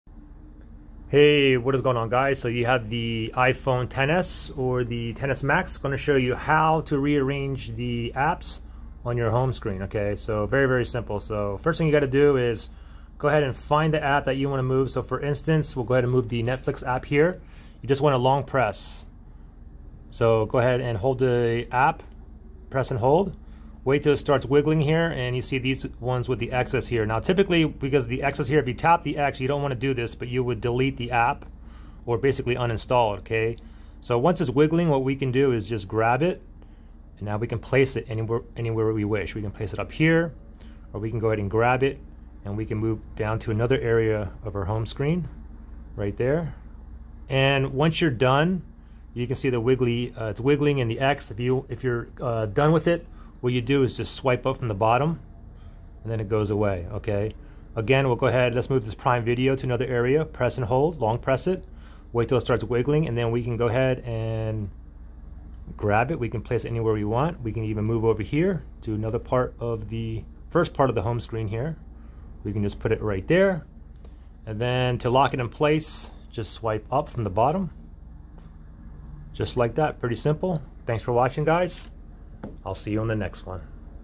iPhone XS / XS Max: How to Rearrange (Move) Apps on Home Screen
I show you how to move or rearrange the apps to a different area of your screen on the iPhone XS and XS Max. Any questions let me know. thx.